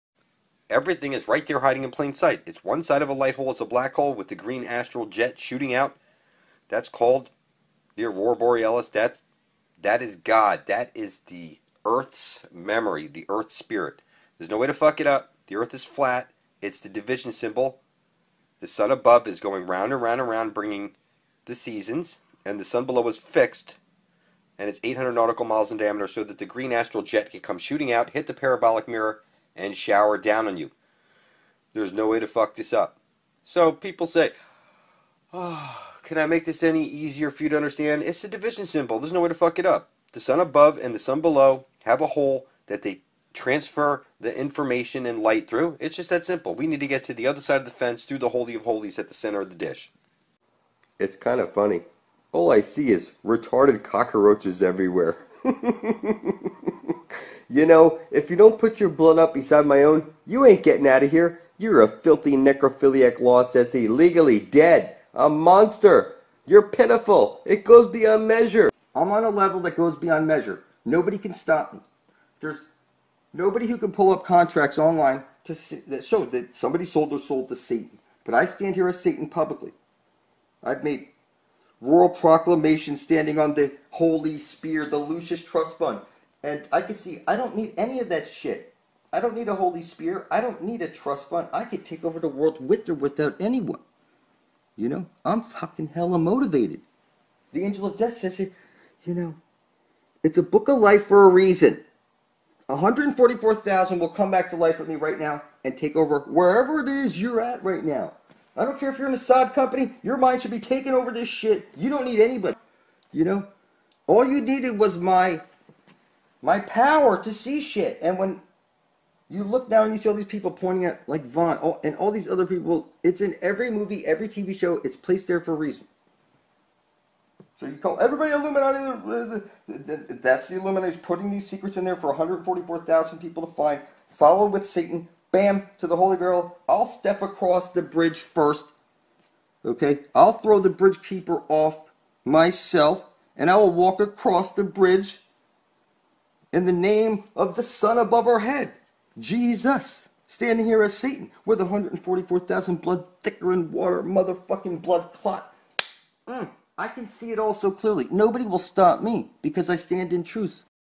Your Birthright
You're supposed to be drinking from the living waters as a super being, but you've been tricked. Get to Blood Over Intent for Heaven on Earth. It's atonement for your soul.